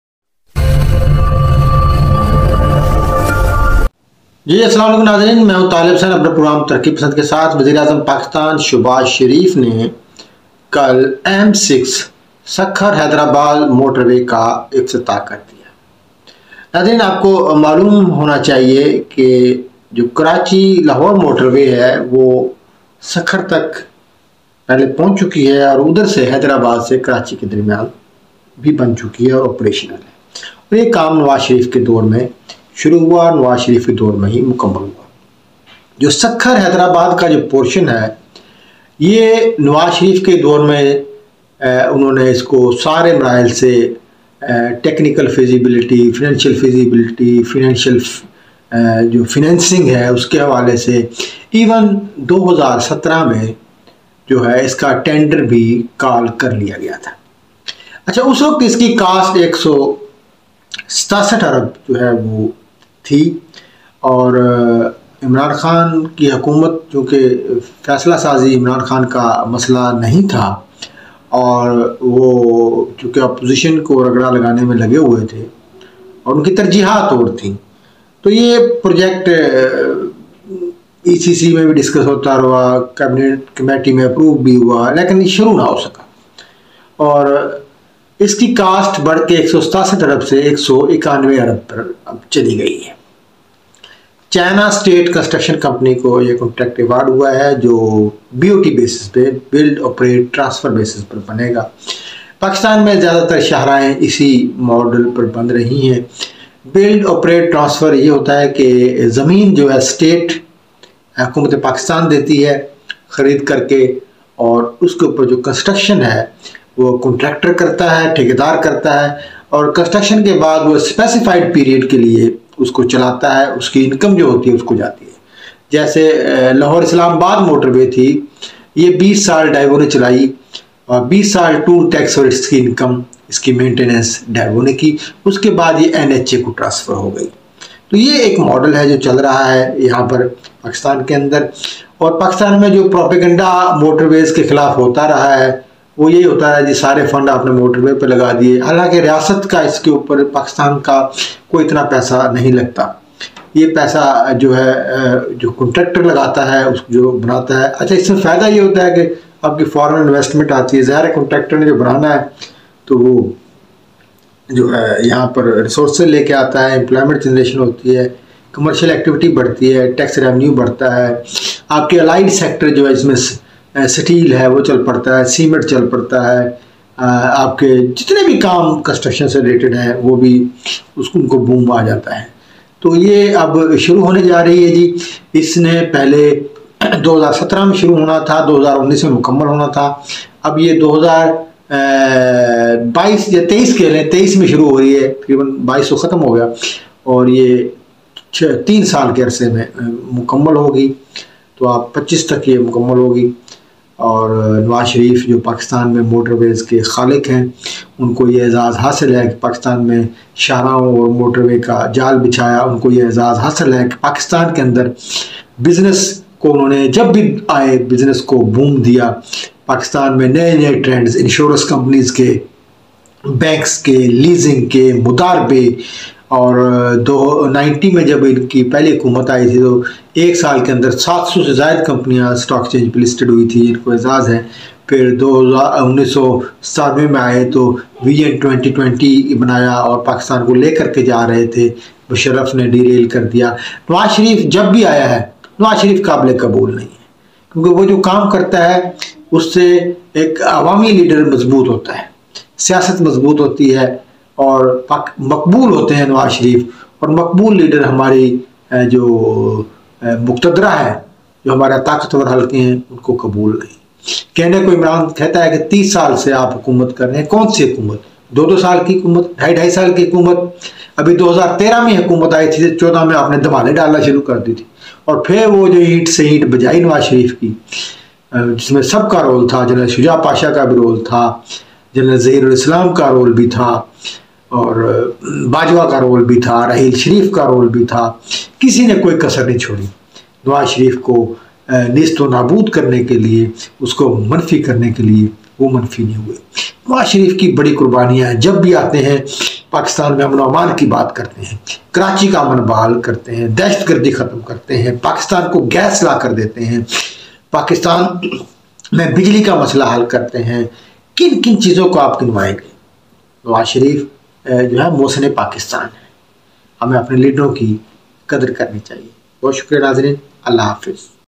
Sukhur Hyderbadad motorway M-6
PM inaugurated M6 motorway